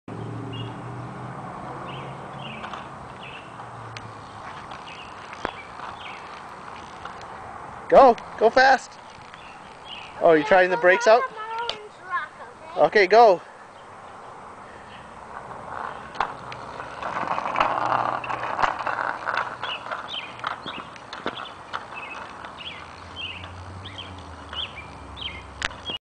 Will's bike
Will riding his first bike